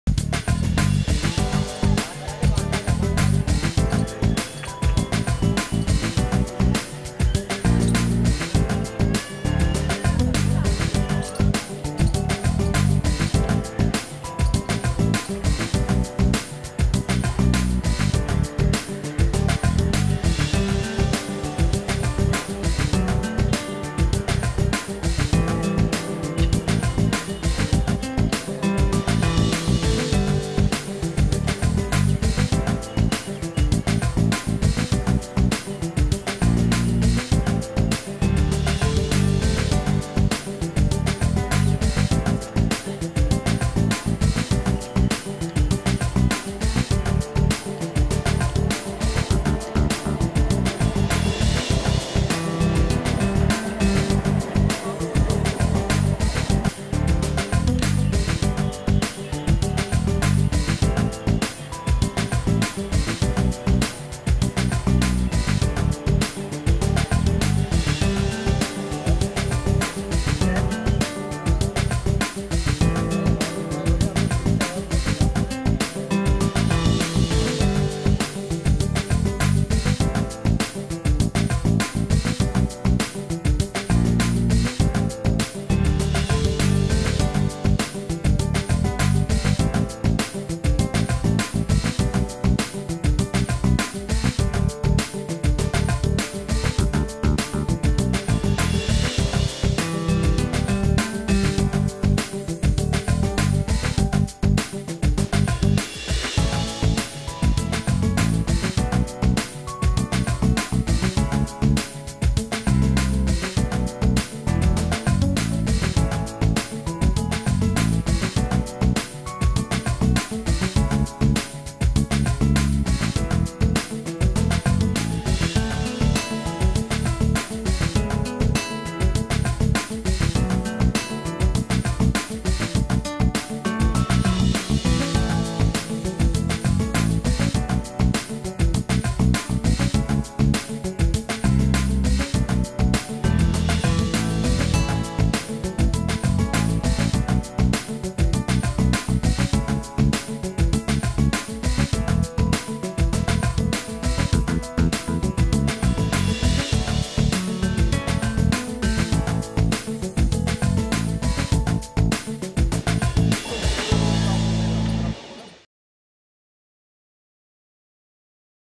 Indian washerman at work on a bank of river Yamuna, Delhi
Indian people collect the clothes on a dhobi ghat near the bank the Yamuna river in New Delhi. They unfold the bundle of dirty clothes A kid can be seen working on a dhobi ghat. One can see the traffic on the iron bridge. They use a traditional way to remove the stain, they keep the clothes overheat and apply chemical on the clothes and cover them.